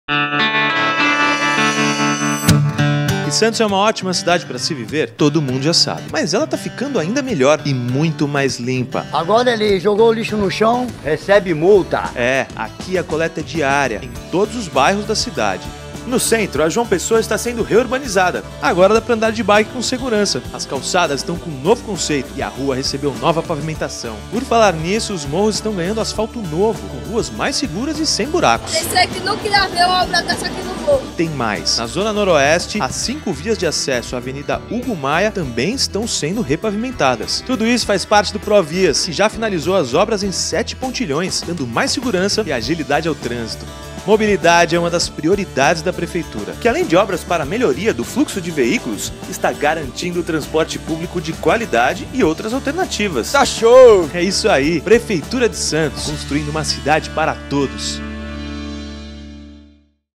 Santos em transformação: mobilidade urbana é uma das prioridades da Prefeitura
Mobilidade urbana é uma das prioridades da Prefeitura. Hoje são 1.800 metros de ciclovia; no total são 65 km de vias repavimentadas, sendo 8 km somente nos morros. Sete pontilhões já foram reconstruídos, após cinco décadas sem manutenção, além do transporte coletivo de qualidade, com frota de ônibus com ar-condicionado e wi-fi.